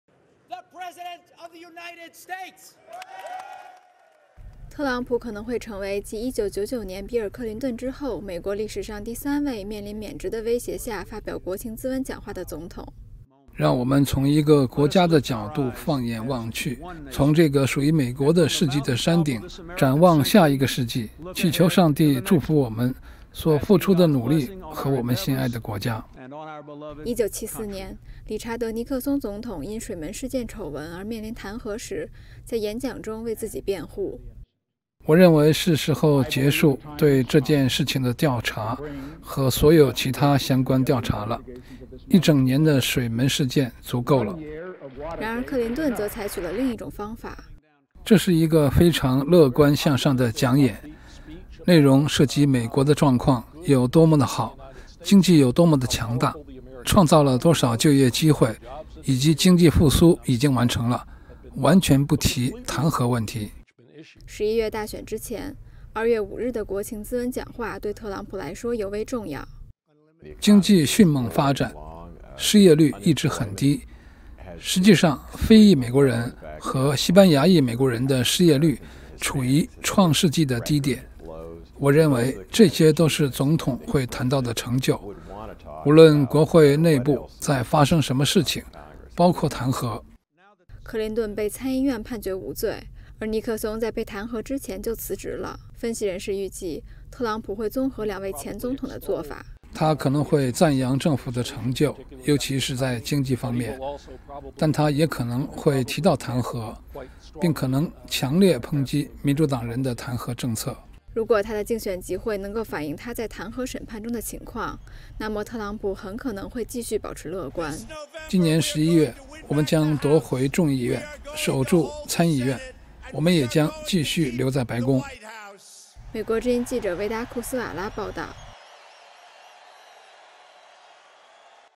特朗普下周二发表国情咨文讲话
对特朗普进行弹劾审判的美国参议院预计本周五将投票表决是否召集证人出庭作证。如果投票通过传唤证人，这意味着特朗普下周二将在被审判的情况下发表年度国情咨文讲话。 美国前总统克林顿和尼克松也曾在面对弹劾的情况下发表国情咨文讲话。